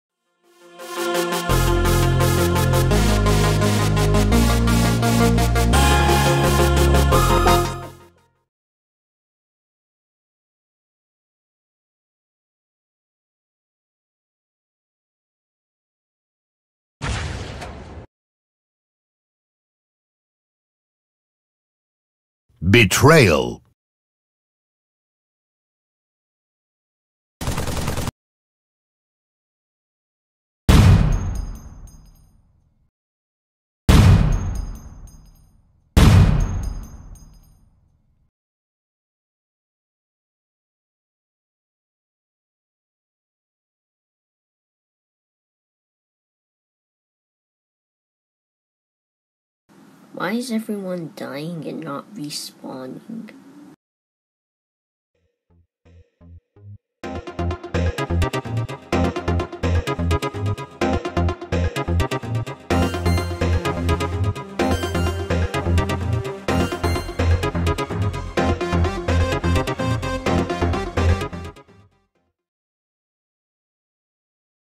Forge- a Halo Mega Bloks/ Construx stopmotion
What happens when chaotic Spartans go into Forge mode?